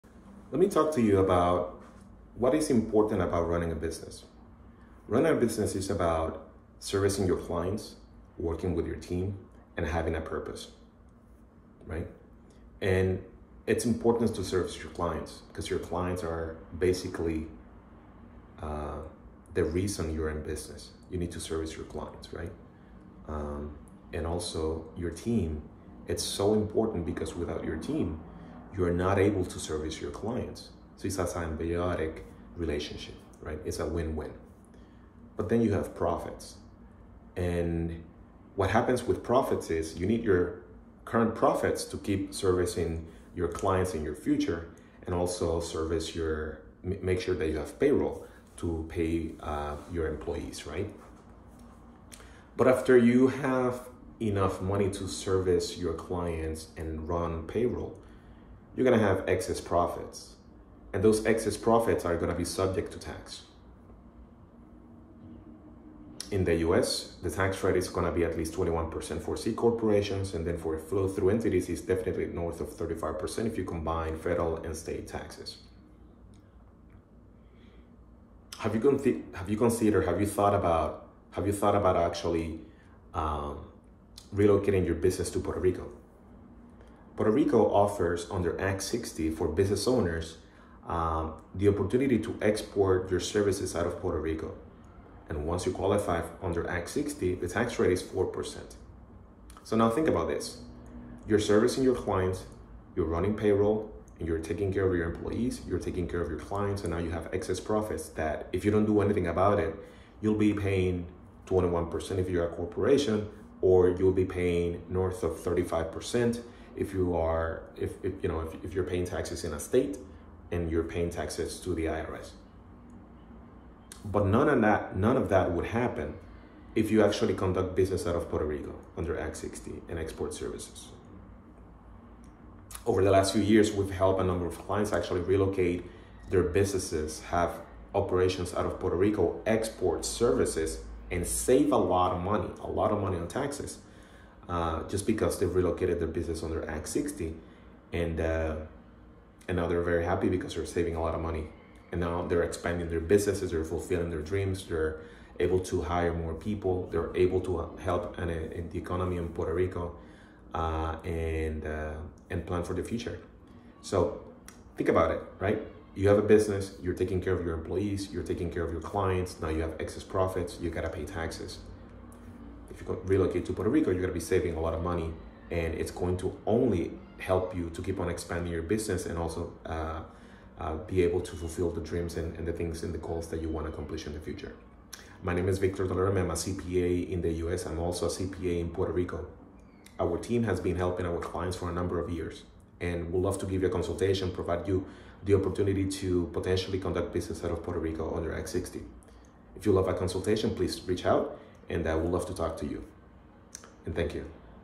Running a Business is Expensive: How to Pay 4% Tax Instead of 21% with Puerto Rico Act 60
Running a business is about more than just making profits—it's about serving your clients, working with your team, and having a purpose. But as your business grows, so do your profits, and those profits are subject to taxes. In the U.S., corporate tax rates can reach 21% for C-corporations and even higher for other business structures, sometimes north of 35% when you factor in federal and state taxes.

What if I told you there’s a way to reduce your tax rate to just 4%? By relocating your business to Puerto Rico under Act 60, you can export your services and enjoy this reduced tax rate. Over the past few years, we’ve helped many clients successfully relocate their businesses, save significant amounts on taxes, and reinvest those savings into growing their businesses. This has allowed them to expand, hire more people, and fulfill their long-term goals.

If you need a Financial/Business Coach or help handling your tax and accounting needs for your small business, contact my office:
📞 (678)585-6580